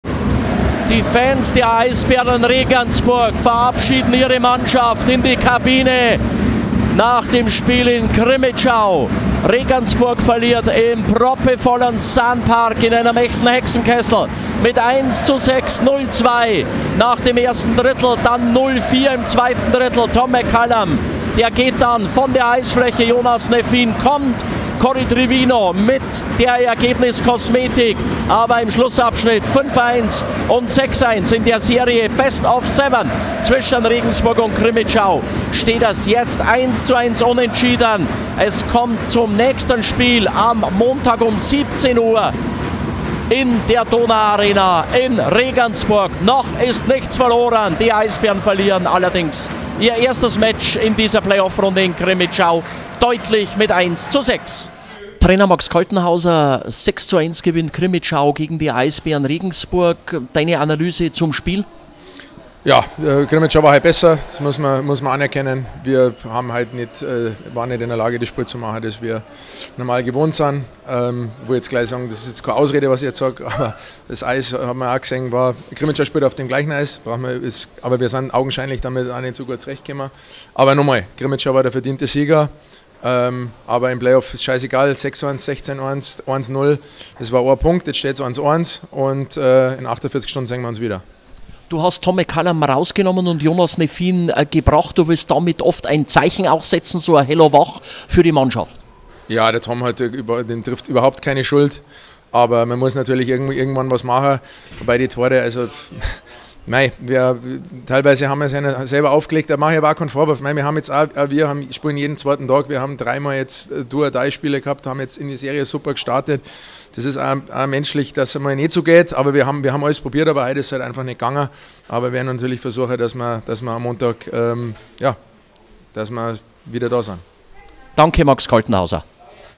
NIEDERLAGE IN CRIMMITSCHAU: EISBÄREN REGENSBURG UNTERLIEGEN EISPIRATEN 1:6 - VIDEO-NACHBERICHT
DEUTLICHE NIEDERLAGE IN CRIMMITSCHAU: EISBÄREN REGENSBURG UNTERLIEGEN EISPIRATEN IN SPIEL ZWEI 1:6 - VIDEO-NACHBERICHT MIT ARMIN WOLF

Das war nicht das Spiel unserer Eisbären: In Crimmitschau setzt es eine 1:6-Niederlage in Spiel zwei des Play-off-Halbfinals...

Armin Wolf war dabei und hat nach dem Spiel mit Coach Max Kaltenhauser gesprochen.

🥅 EBR-TORSCHÜTZE 🥅
4:1 Corey Trivino (40. Minute)

Morgen Vormittag geht auf unserer offiziellen EBR-Homepage auch der schriftliche Nachbericht online, der euch auch zu den Highlights und zur PK führt:

Damit schafften die Sachsen im zweiten Spiel der „Best of Seven“-Halbfinalserie den 1:1-Ausgleich. Es geht direkt weiter: Schon am Montag, 1. April, steht die dritte Begegnung auf dem Programm, die dann wiederum in der Donau-Arena stattfinden wird. Spielbeginn ist ob des Feiertags bereits um 17 Uhr. Die Plätze in der Arena sind heiß begehrt, es gibt nur noch wenige Karten.

Damit steht fest: Es wird mindestens Spiel fünf in der Serie geben - dieses findet am Freitag, 5. April, ab 20 Uhr in der Donau-Arena statt. Der Vorverkauf für diese Partie startet am morgigen Ostersonntag, 31. April, um 10 Uhr vormittags.

Die Nachberichte werden präsentiert von Kullman's Grill & Diner in Regensburg!

Foto: Tommy Castro